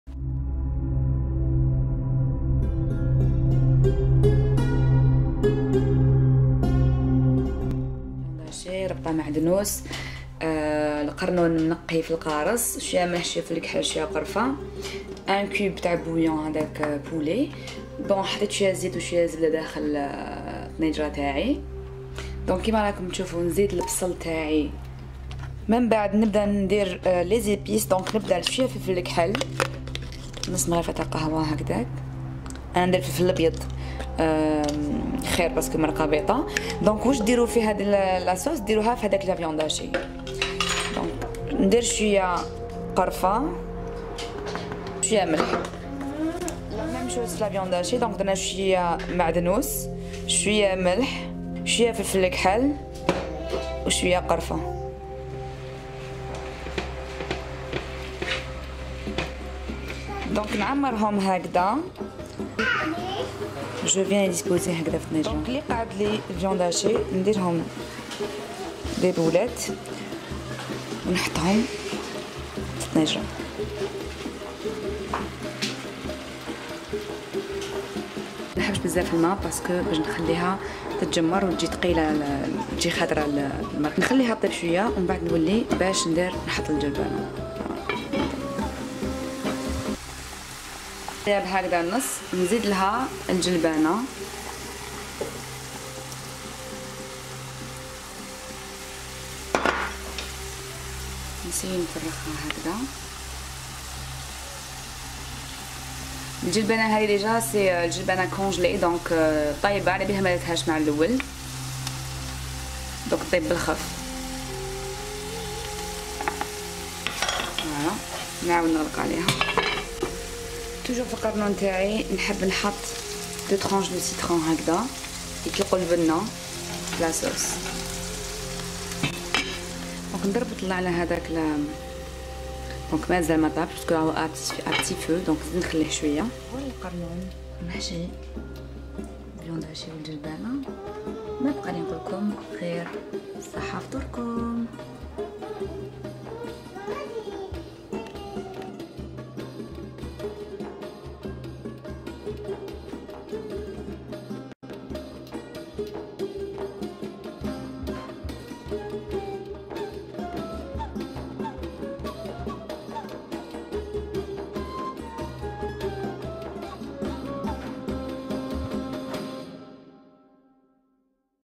Recette KARNOUN MAHCHI Artichauts au petits pois dz
Ingredients:

400 g de viande d'agneau (moi je n'en ai pas mis car je trouve que c'est trop de viande )
400g de petits pois
500g viande hachée
6 Artichauts
1  oignon râpé
2CS D'huile végétale
1CS de margarine ou bien smen pour la recette originale
1cc de cannelle (bâton de cannelle c mieux pour une sauce blanche moi j'en avait pas ce jour là )
1cc de poivre blanc
1CC Sel
Persil
Citron
Un cube bouillon (facultatif)

Bon appétit les filles❤️❤️❤️❤️

morena_zen

Je filme avec une Camera Canon EOS 700D
Je fais mes montages avec Movie Maker